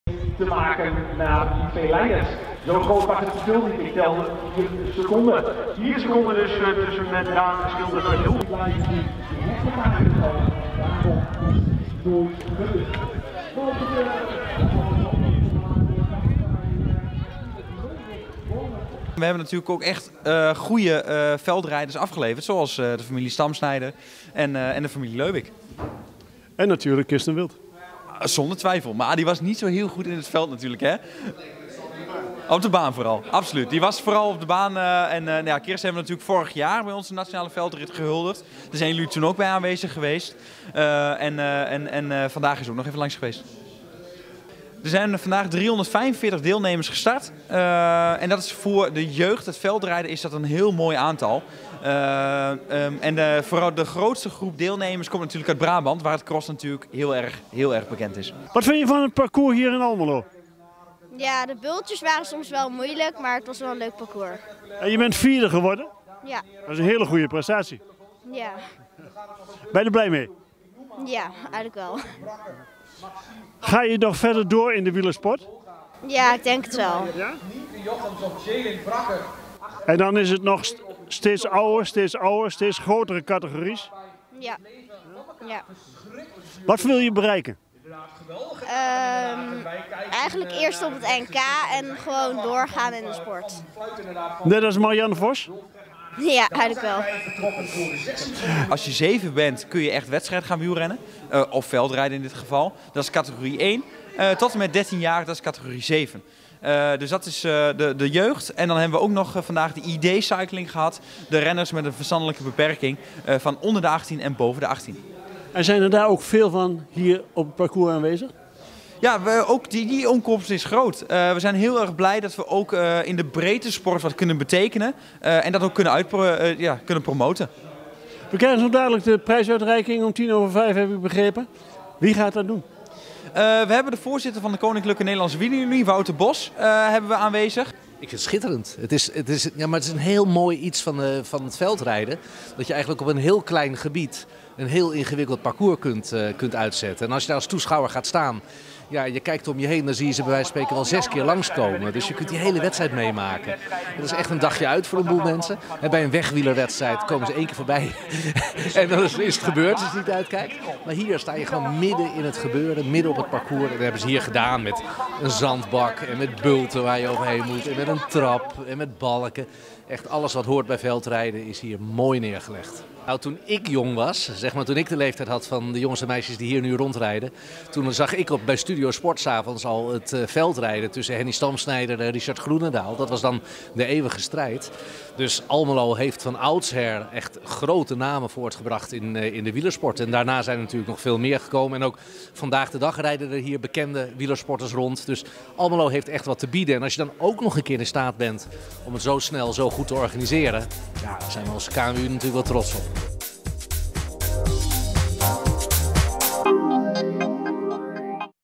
NK Veldrijden jeugd op De Sumpel
Afgelopen weekend was AWV de Zwaluwen gastheer van de Nederlandse kampioenschappen veldrijden voor de jeugd. Rond hun clubparcours De Sumpel was het dan ook een drukte van belang. Ruim driehonderd veldrijders schreven zich in voor dit evenement. Maar liefst zeventien verschillende titels waren er te vergeven.